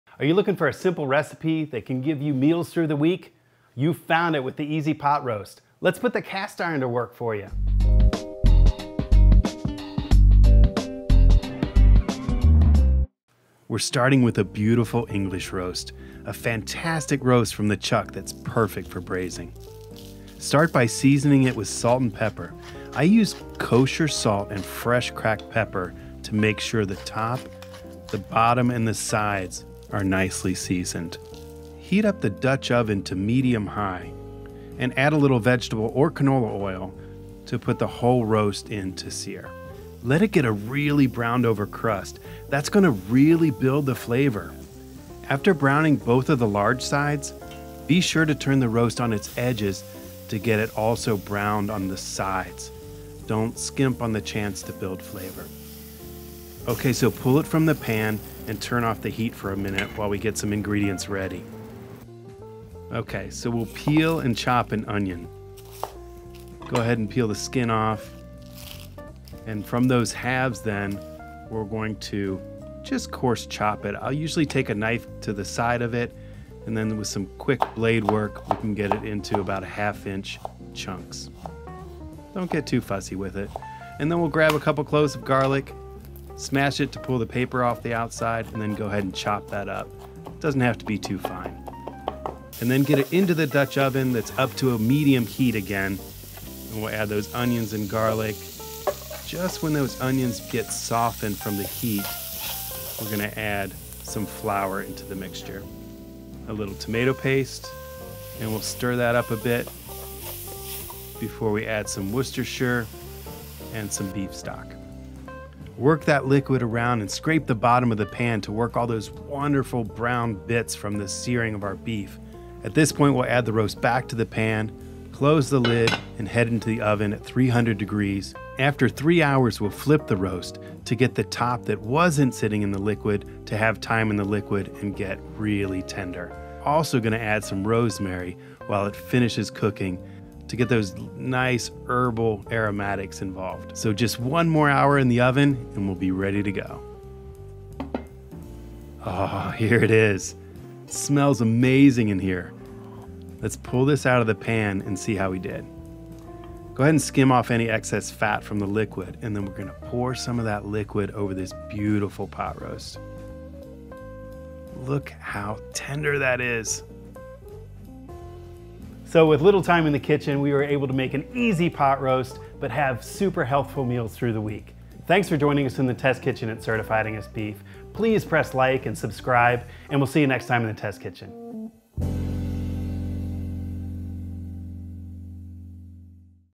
Easy Dutch Oven Pot Roast Recipe
Need a beefy "comfort food" kind of meal? Make this EASY POT ROAST recipe. You'll need an English roast (chuck roast), onions, garlic and a couple of staple ingredients. Here's how!

INGREDIENTS:
3 1/2 pounds Certified Angus Beef ® Chuck Roast or English roast
1 tablespoon coarse kosher salt
2 teaspoons freshly ground black pepper
2 tablespoons vegetable oil
1 onion, chopped
2 cloves garlic, chopped
1 tablespoon flour
1/4 cup tomato paste
2 cups beef stock
2 tablespoons Worcestershire sauce
1 sprig rosemary